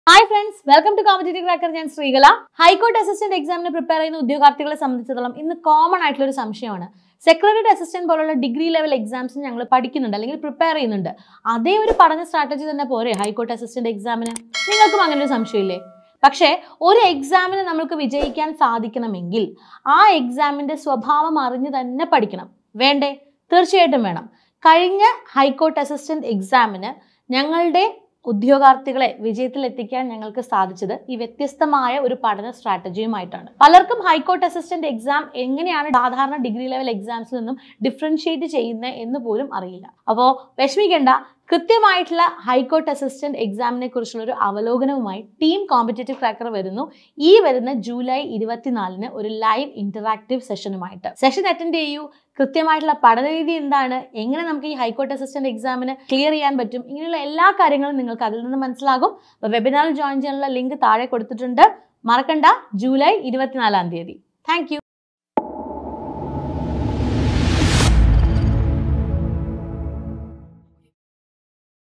HIGH COURT ASSISTANT എല്ലാം വ്യക്തമാണോ ?
High Court Assistant പരീക്ഷയ്ക്ക് സെക്രട്ടറിയേറ്റ് പ്രാഥമിക പരീക്ഷ പോലെ പഠിച്ചാല്‍ വിജയിക്കാന്‍ പറ്റുമോ? നോട്ടിഫിക്കേഷന്‍ വന്നതുമുതല്‍ ഈ സംശയത്തോടെയാണ് ഉദ്യോഗാര്‍ത്ഥികളില്‍ ഒരു വിഭാഗം. എങ്ങനെയാണ് ഹൈക്കോര്‍ട്ട് അസിസ്റ്റന്റ് പരീക്ഷയെന്ന് അറിയാത്തവരുമുണ്ട്. പരീക്ഷയുടെ തയ്യാറെടുപ്പുകള്‍ തുടരട്ടെ, പക്ഷെ പരീക്ഷയുടെ സ്വഭാവമറിയാതെ തെറ്റായ രീതിയില്‍ പഠനം തുടങ്ങരുത്. ശരിയായ പഠനരീതിയും ടൈംടേബിള്‍ എങ്ങനെ തയ്യാറാക്കാമെന്നുമറിയാന്‍ ജൂലൈ 24ലെ സെഷനില്‍ പങ്കെടുക്കൂ.